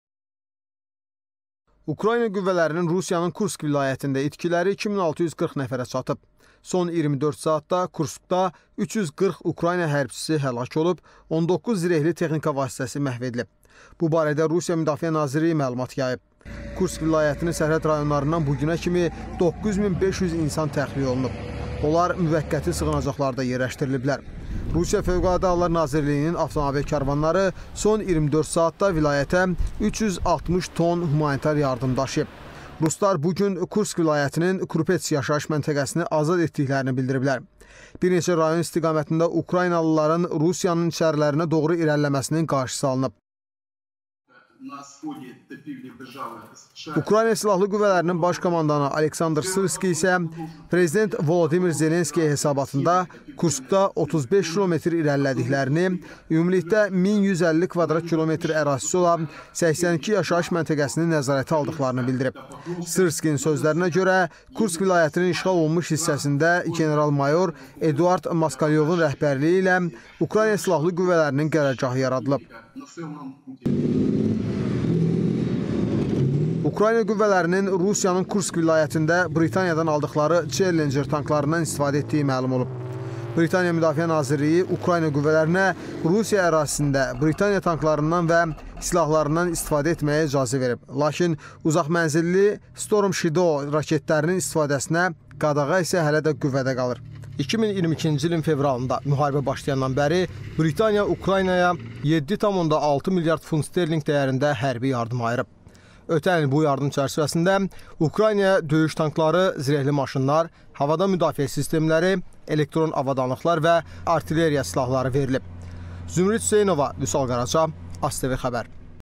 Şiddətli döyüşlər gedir, minlərlə insan təxliyə edilir: 83 ildir Rusiya belə hadisə yaşamamışdı
KURSKDA QAN SU YERİNƏ AXIRUKRAYNA GÖRÜN NƏ QƏDƏR İTKİ VERDİ